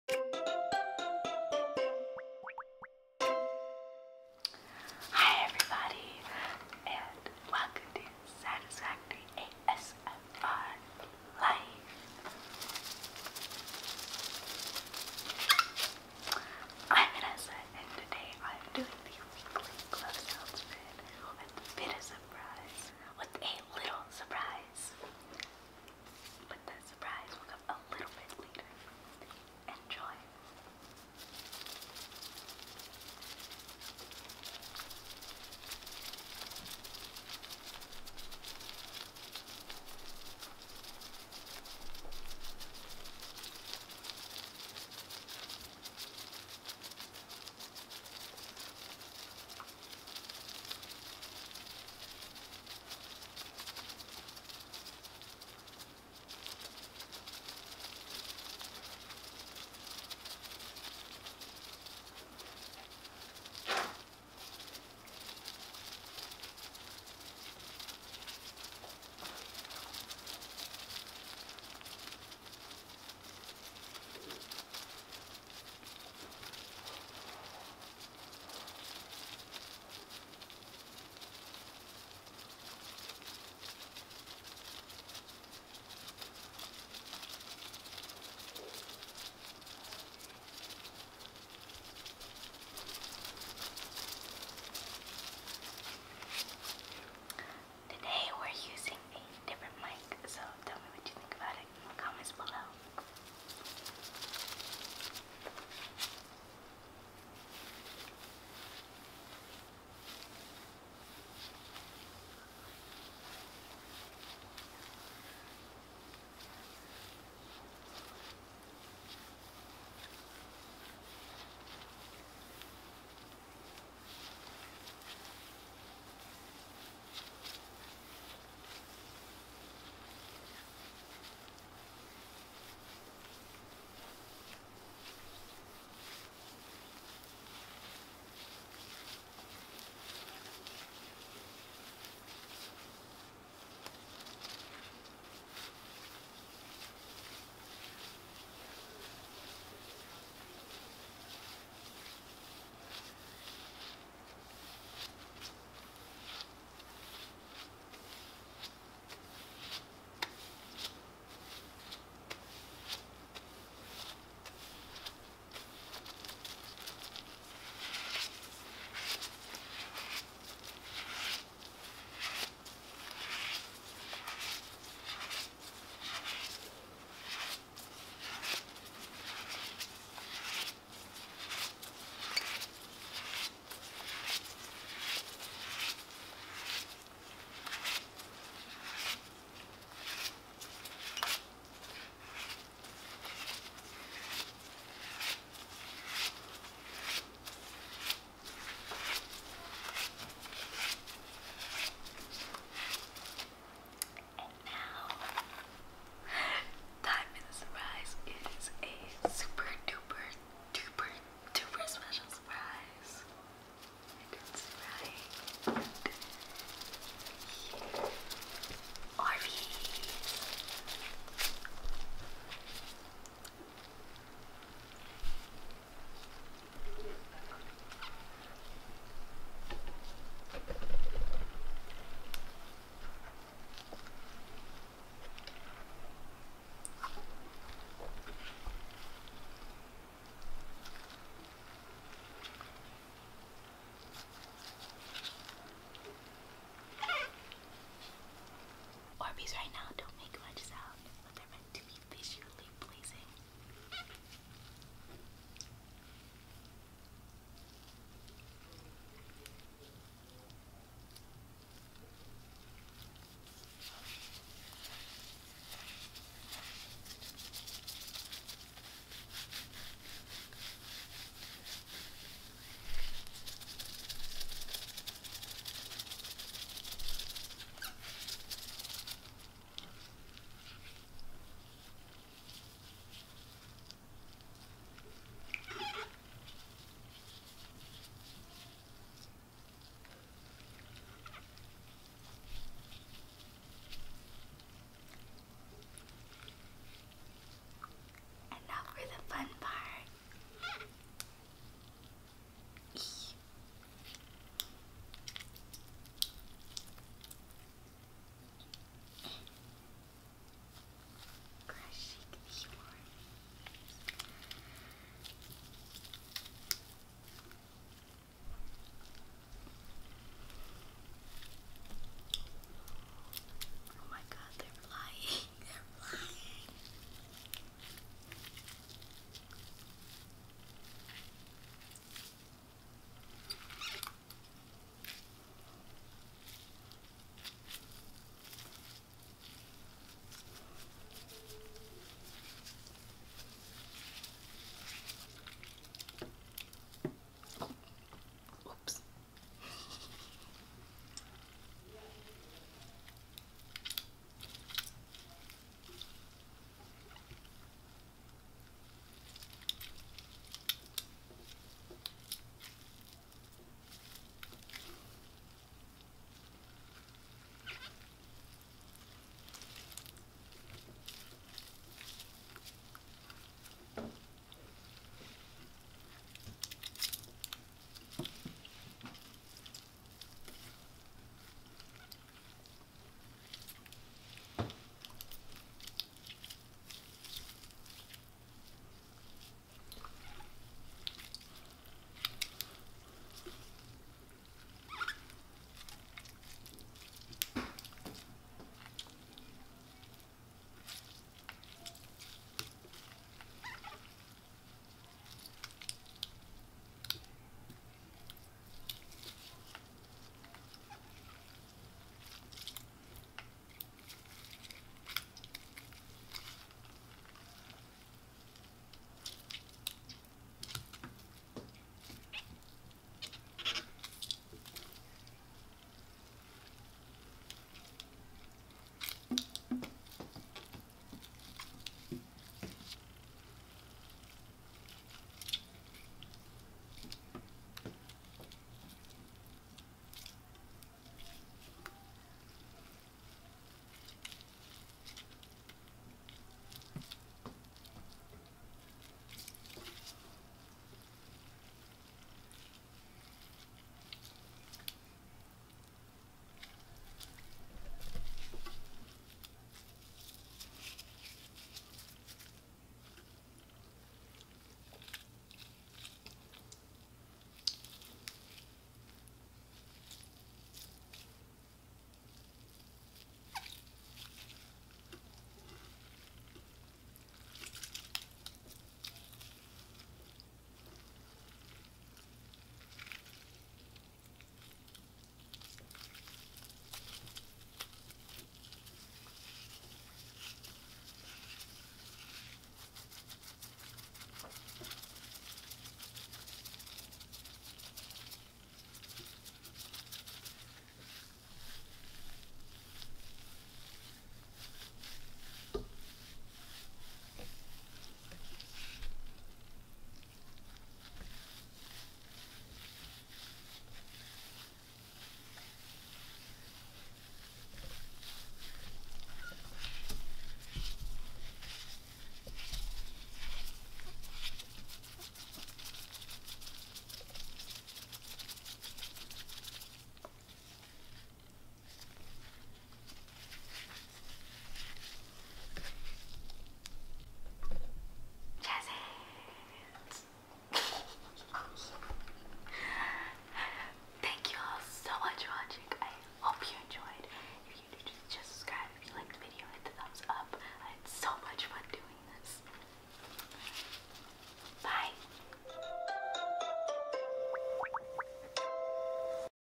ASMR 🧤Latex Gloves 🧤Hand Sounds & Orbeez
Hi Tingle Team,
Welcome to my weekly glove sounds video. This week I will not just be making hand sounds but I will also be playing with Orbeez. There is a surprise towards the end Orbeez crushing that is very satisfying.

Thank you all for all of your love and support! Love you guys!

☆　 FOLLOW ME 　☆

Follow on Twitter @VanessaASMRLife
Follow on IG: SatisfactoryASMRLife